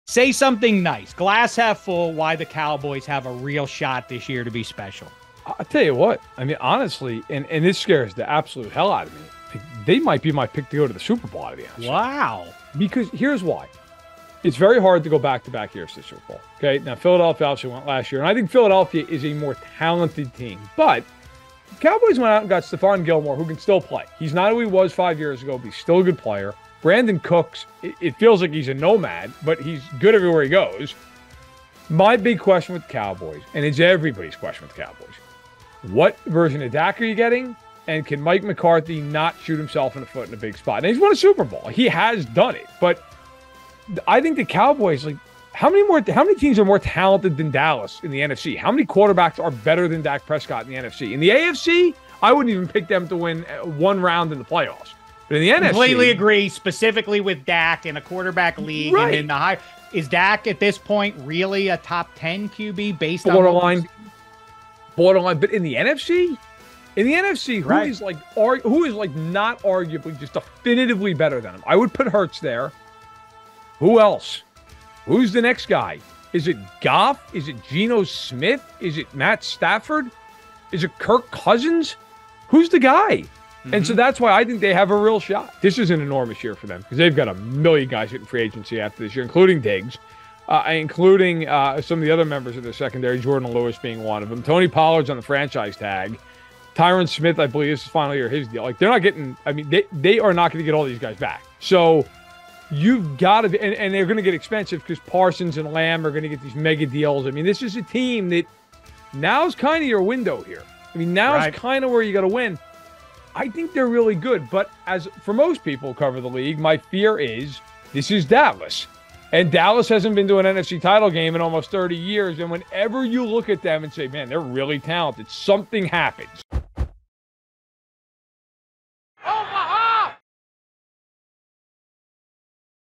The very early prediction of who wins the NFC next season | Minus Three
Dave Dameshek and Matt Verderame preview the entire NFC and Matt gives his pick of who will represent the conference in the Super Bowl